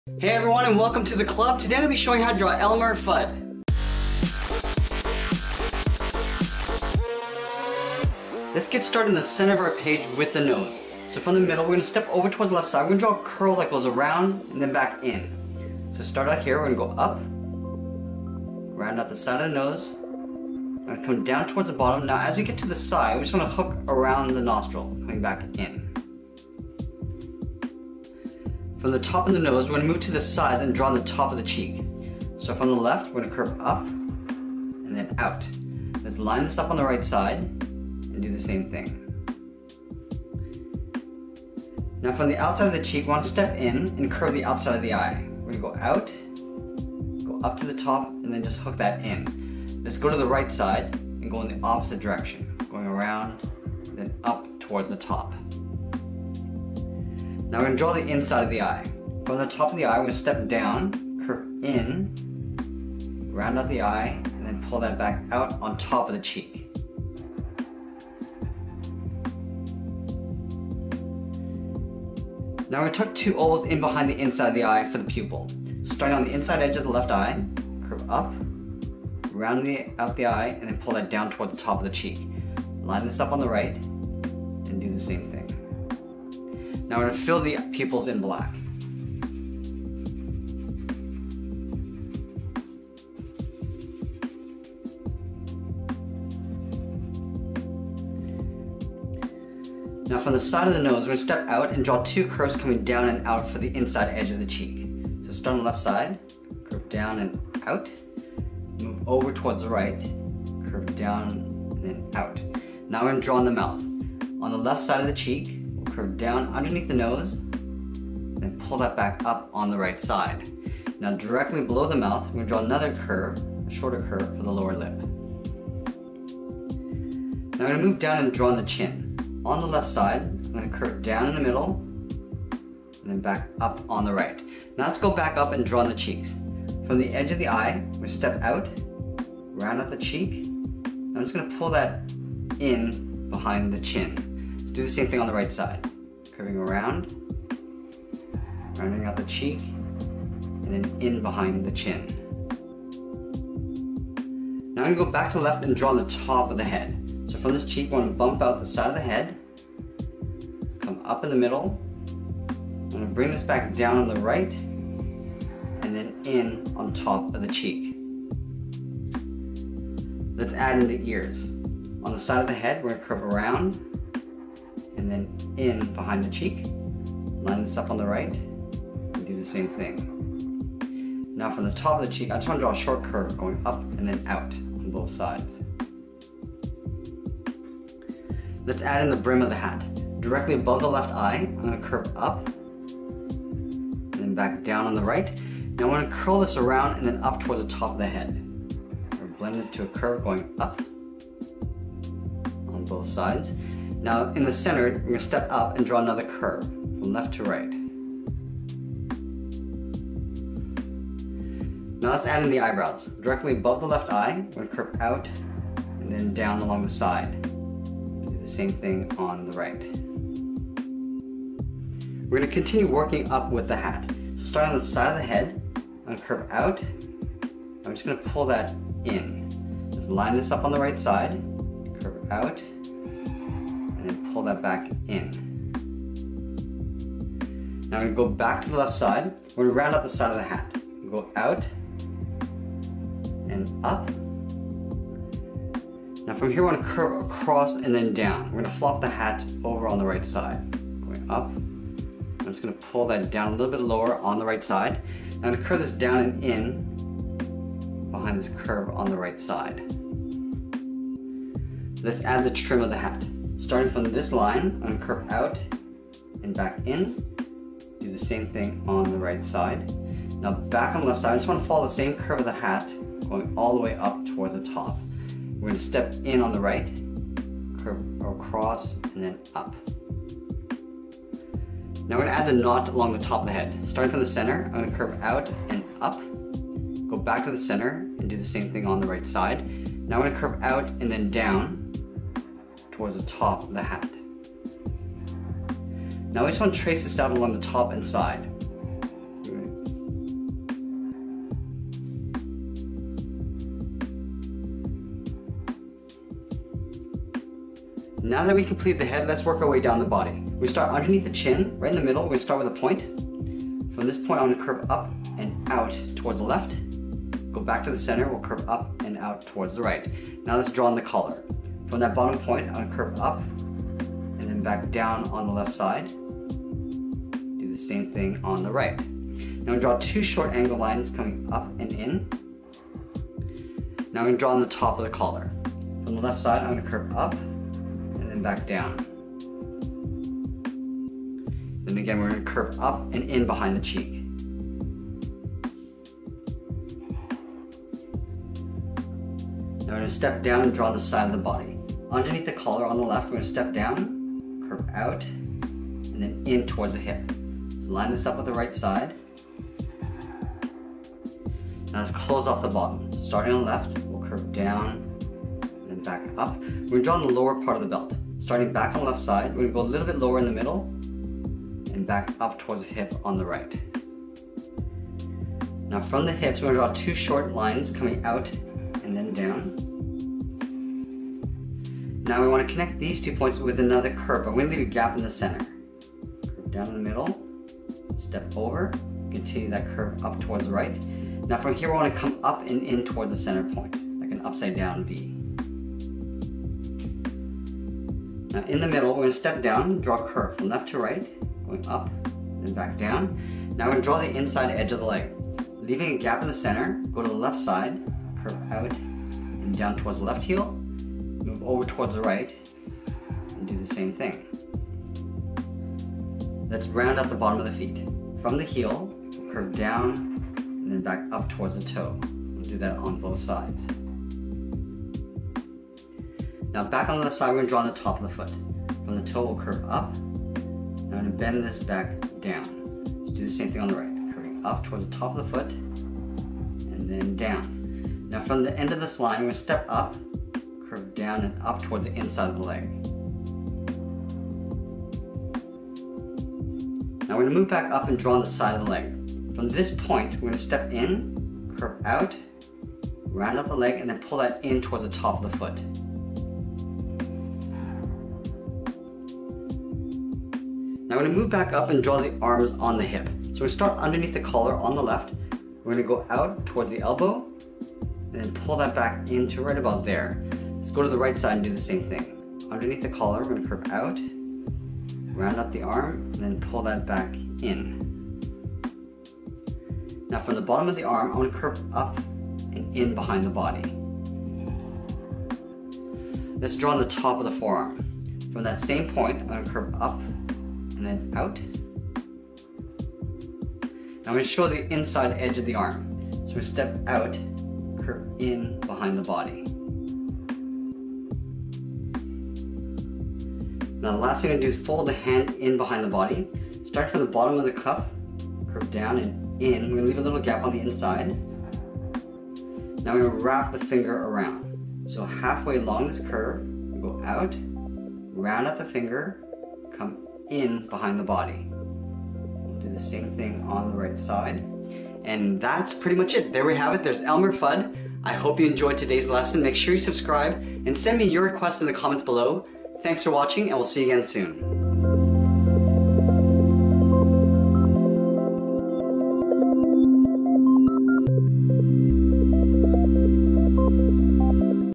How to Draw Elmer Fudd
Learn How to Draw Elmer Fudd. Grab your pencil and paper and watch as I guide you through these easy to follow drawing instructions. Check out my Cartoon Characters Playlist for more of your favourite characters.

Share your art with me on Instagram!!!